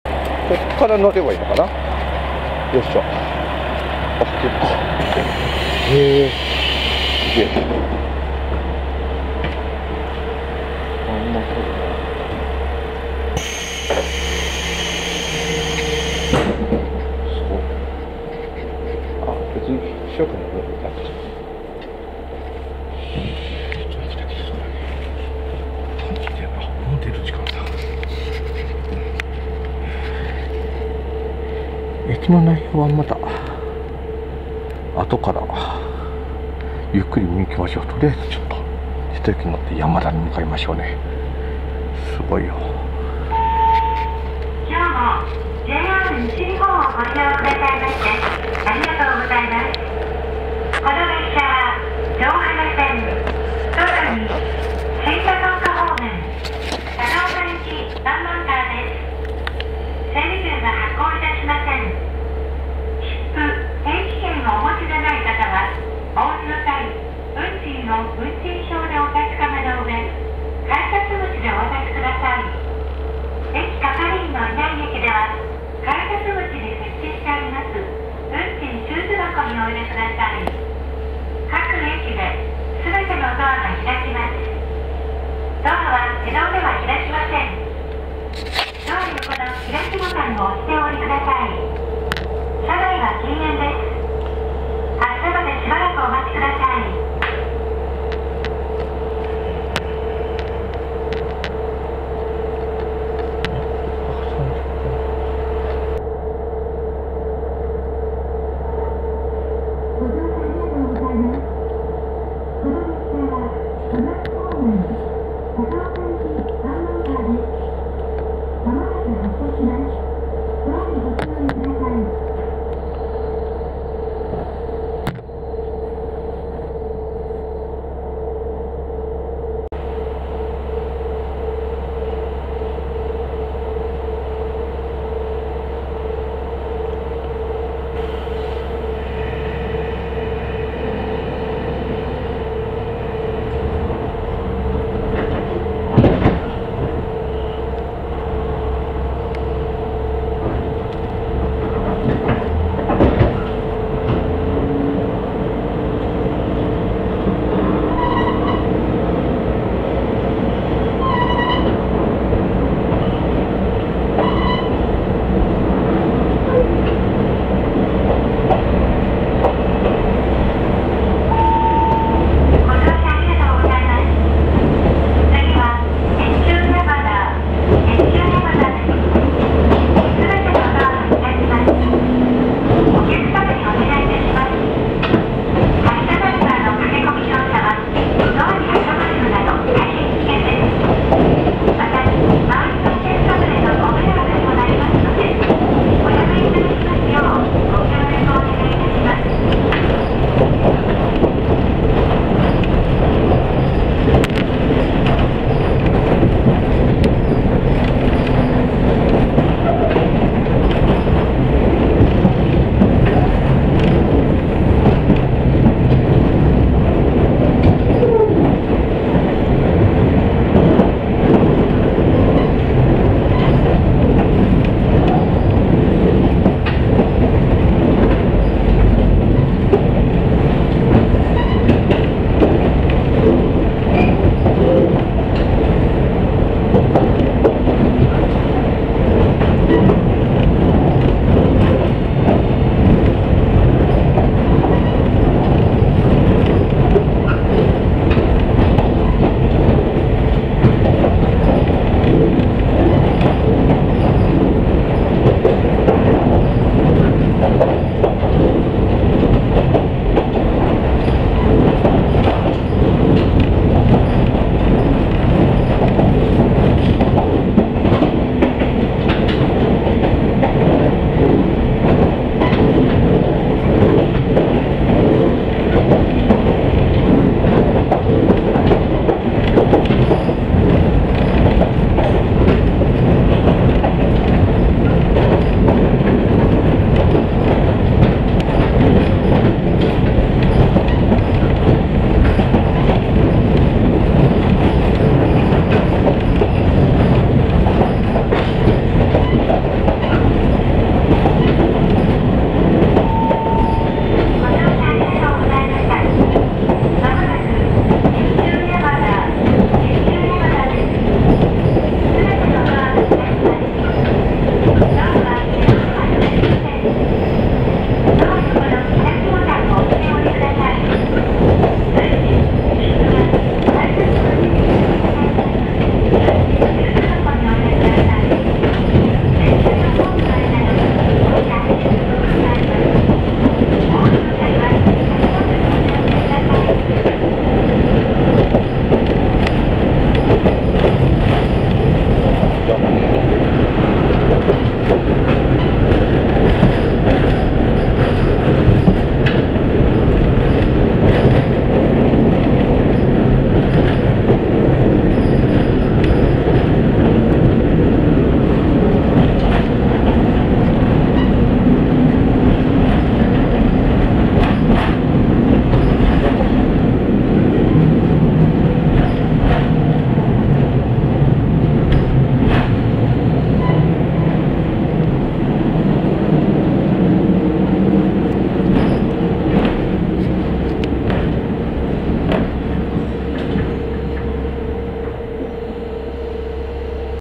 ST130 ep25'1 山田の車窓から：JR城端線・城端駅→越中山田駅【JR西日本ターミナル駅完全制覇の旅～北陸編～】
〒939-1852 富山県南砺市是安
城端駅

関連動画はこちら↓
ST105-5　あまりにも早く網野駅に到着したので、京丹後市網野エリアを早朝ドライブ【由良ゆら巡る・京都丹後鉄道ターミナル駅完全制覇の旅】